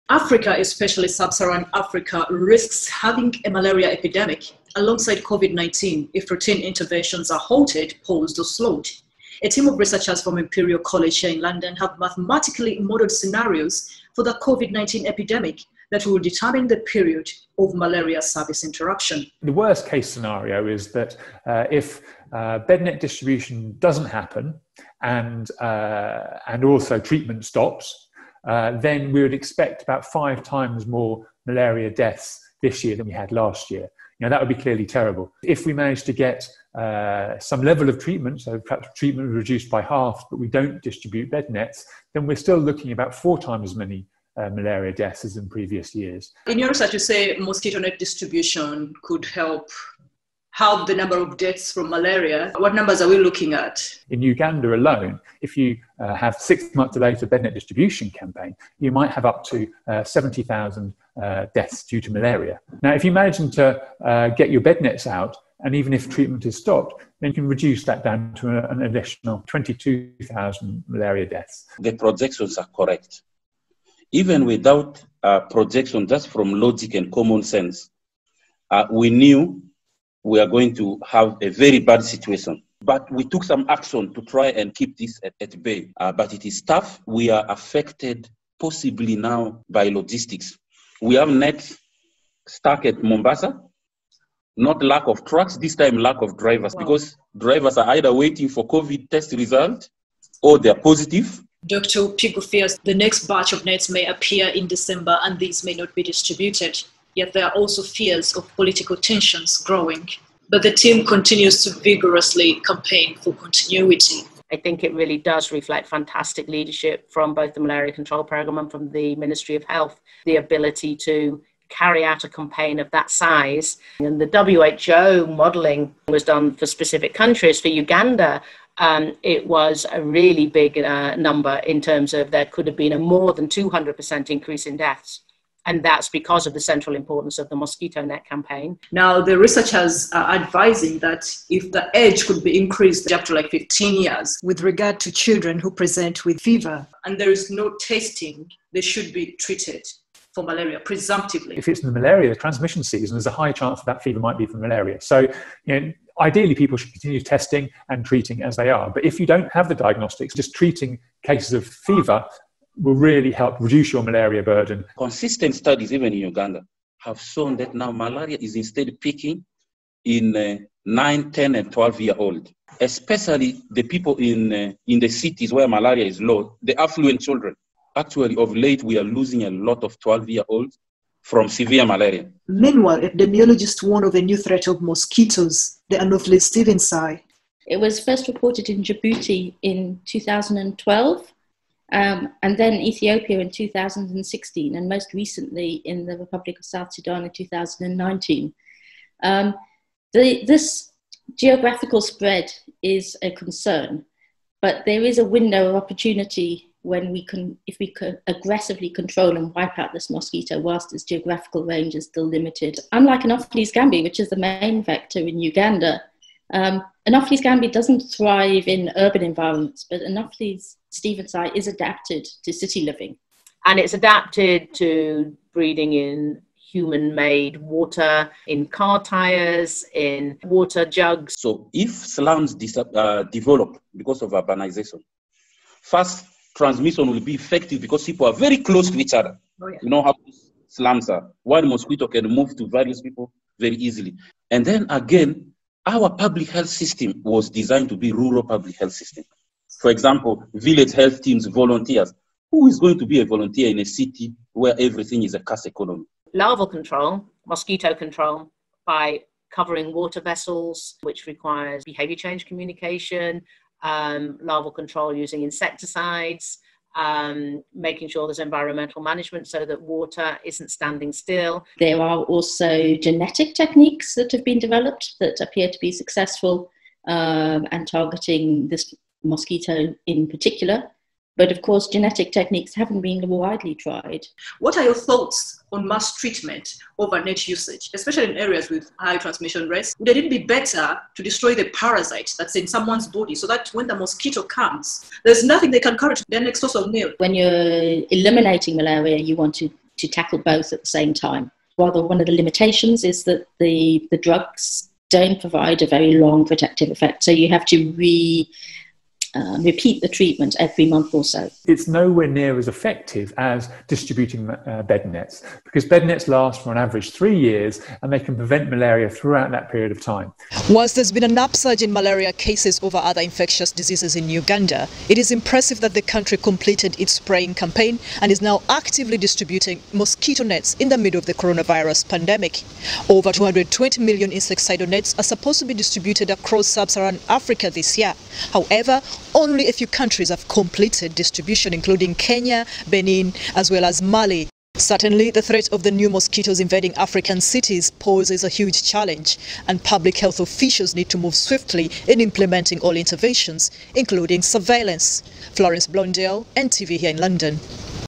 How COVID-19 lockdowns hit malaria fight
According to UK researchers, it has been estimated that because of COVID-19, 780,000 people in Africa will die of malaria this year if nets are not distributed and fewer cases are treated.

In Uganda, the number would be almost 70,000. Amidst all this, the Malaria Centre at the London School of Hygiene and Tropical Medicine warns of a potential threat of urban malaria.